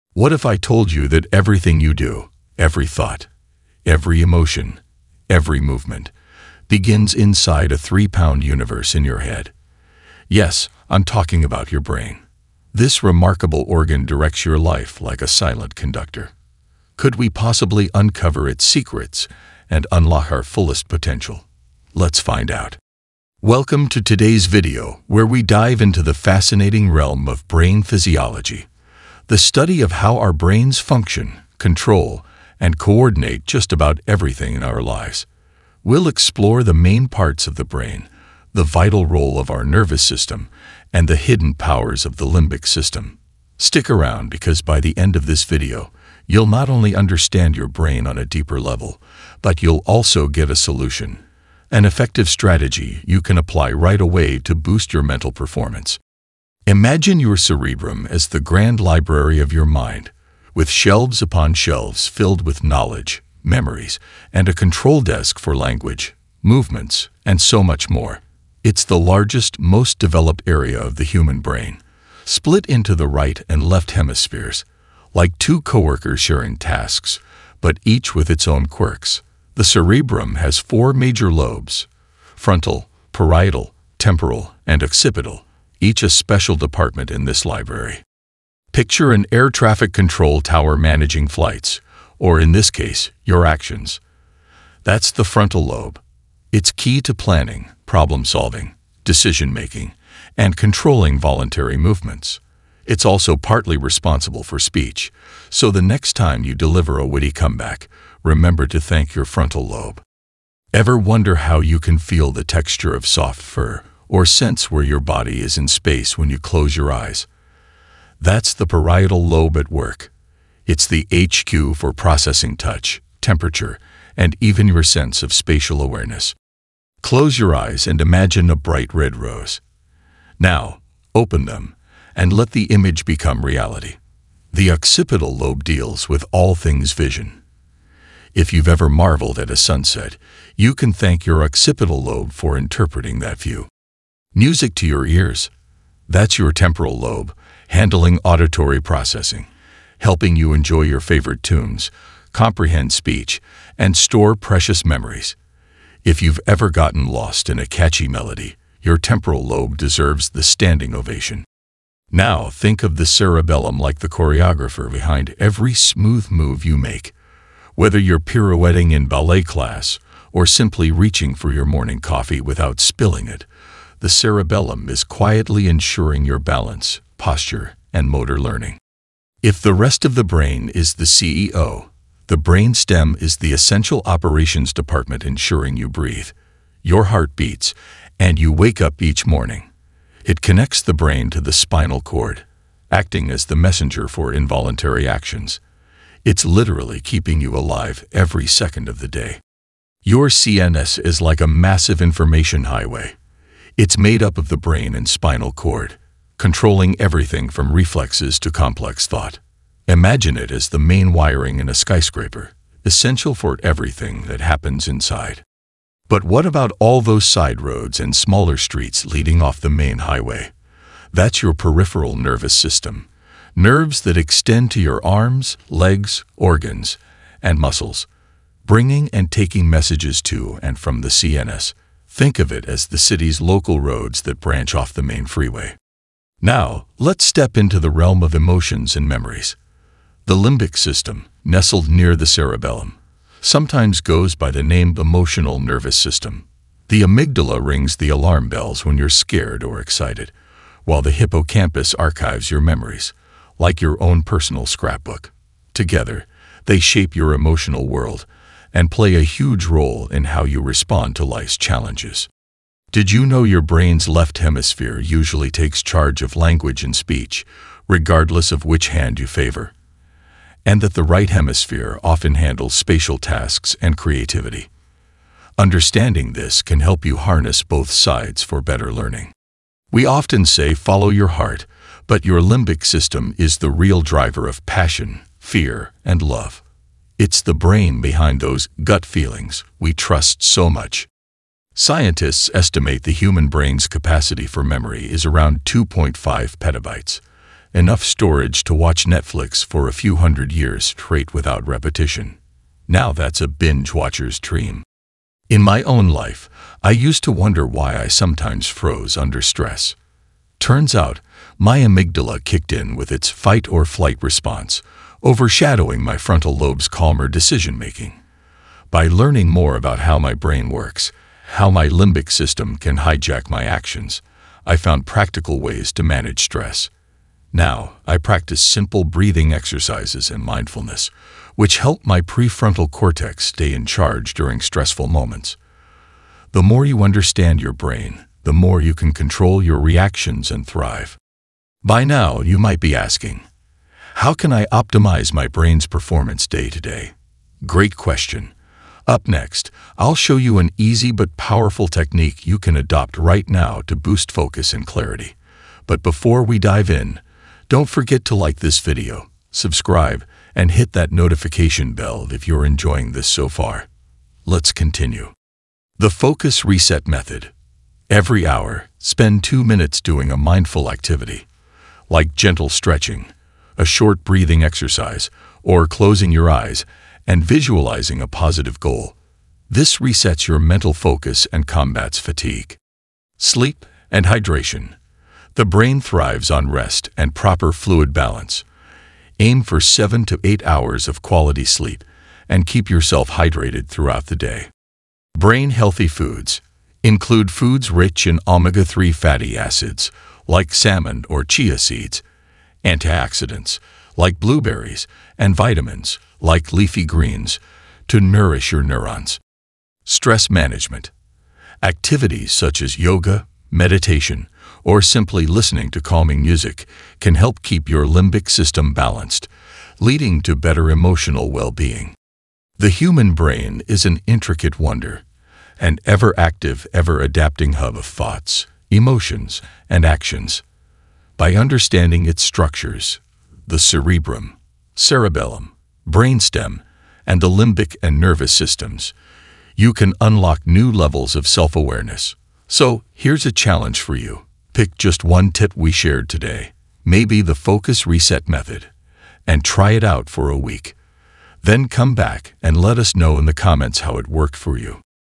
Your Brain Is Playing Tricks on You! Brain Physiology
Unlock the full potential of your mind with our in-depth exploration of brain physiology.

In this video, we'll cover:

Understanding Brain Physiology: Learn how your brain's structure influences your daily life.

Key Brain Components: Discover the roles of the cerebrum, cerebellum, and brain stem.

The Nervous System: Uncover how the central and peripheral nervous systems interact.

The Limbic System: Explore the emotional center of your brain and its impact on behavior.

Practical Tips: Implement strategies to enhance your brain's performance and overall well-being.

By the end of this video, you'll have actionable insights to improve your mental performance and a deeper appreciation for the complexities of your brain.
Brain optimization

Improve mental performance

Brain health tips

Understanding brain function

Enhance cognitive abilities